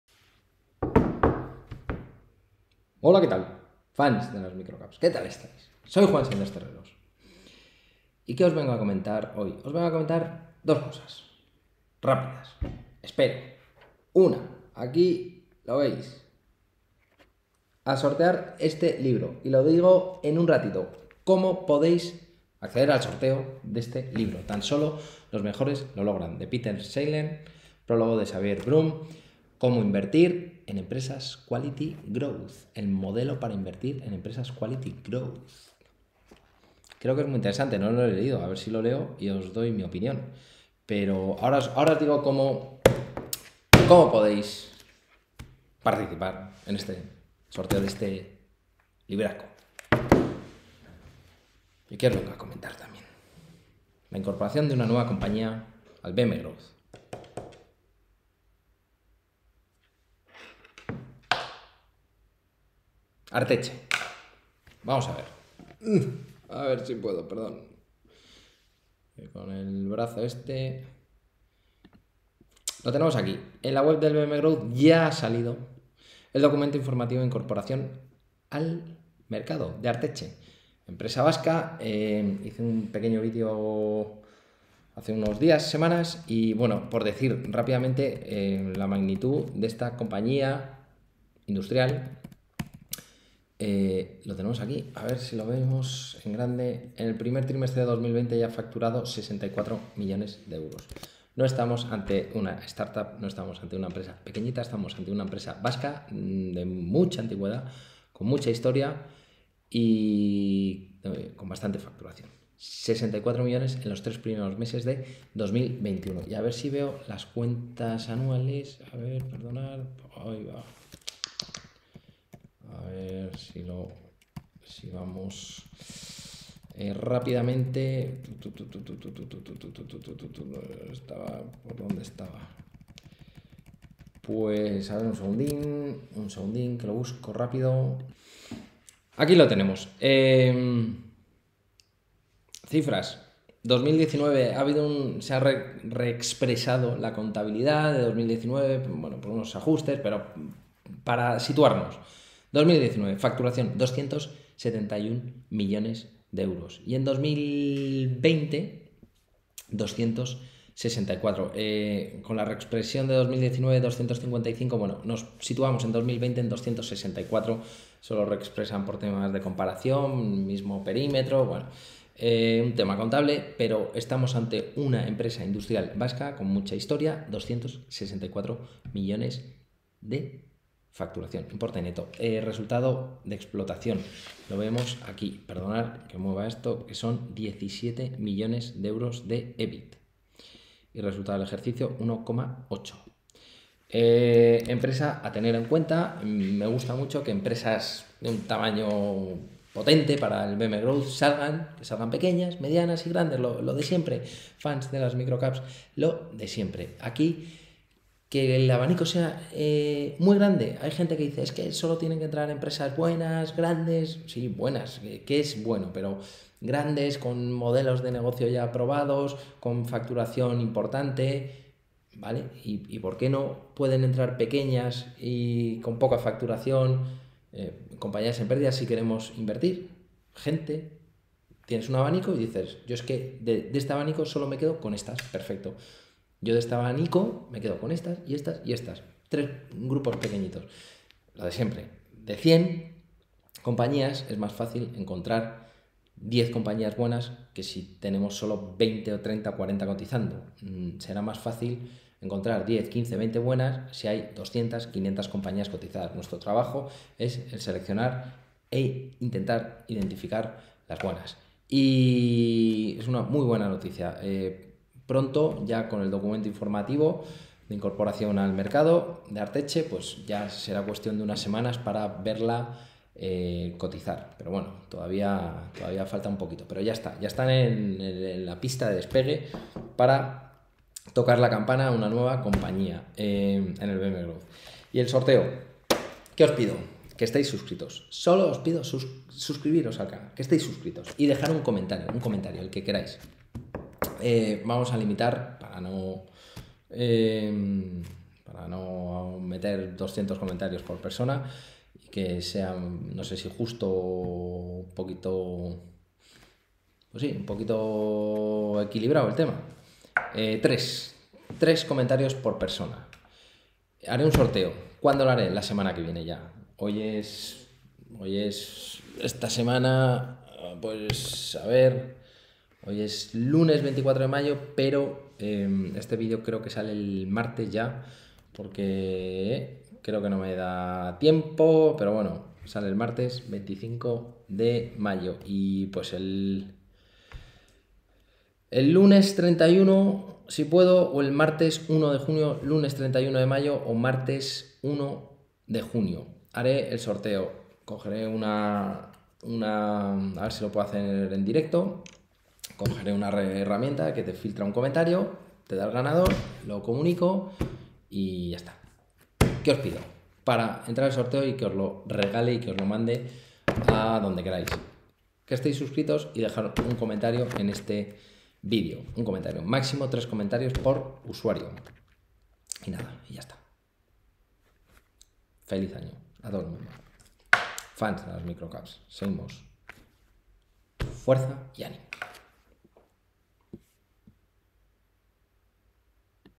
⭐️ QUALITY GROWTH 📈 Un libro + Una filosofía de inversión
De nuevo hago un sorteo de un libro. En esta ocasión se trata del libro "Tan solo los mejores lo logran: La mejor forma para invertir en empresas quality growth" (libro de Peter Seilern).
El sorteo lo haré el lunes 31 de mayo o el martes 1 de junio de 2021.
Sólo tenéis que estar suscritos al canal y dejar un comentario (hasta un máximo de tres por persona).
¡¡Suerte!!